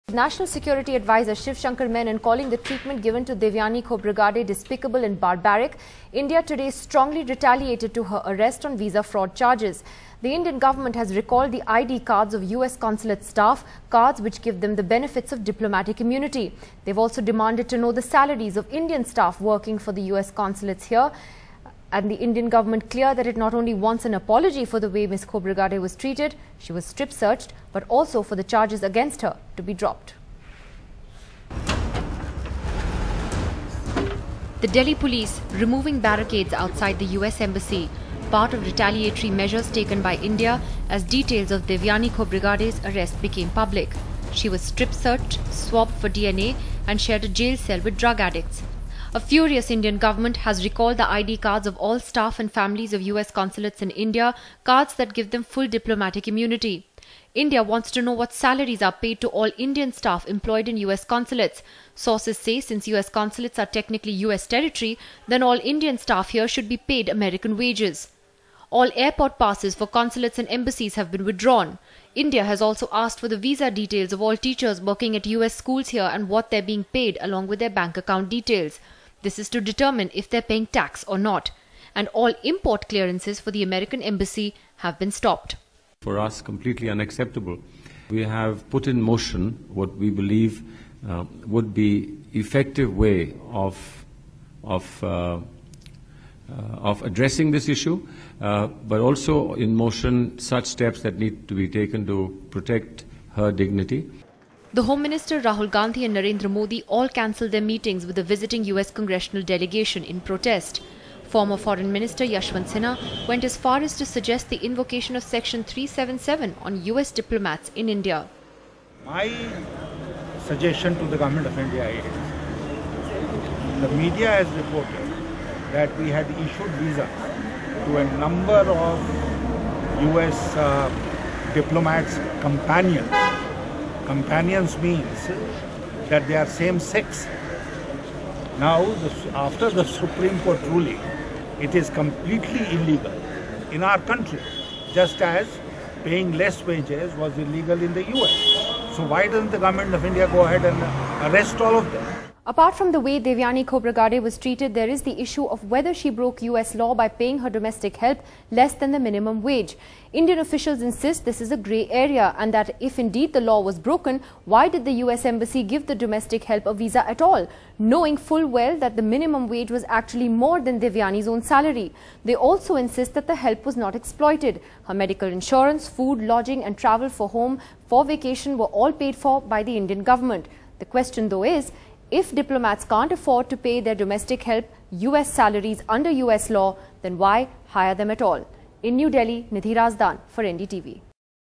Diplomat Devyani Khobragade's arrest: India acts tough with US, takes tit-for-tat measures
The Delhi Police removed security barricades from outside the sprawling US embassy in the capital today, as a furious India retaliated with a slew of measures to pare down the privileges of American diplomats after details of the arrest of its diplomat Devyani Khobragade in New York last week became public.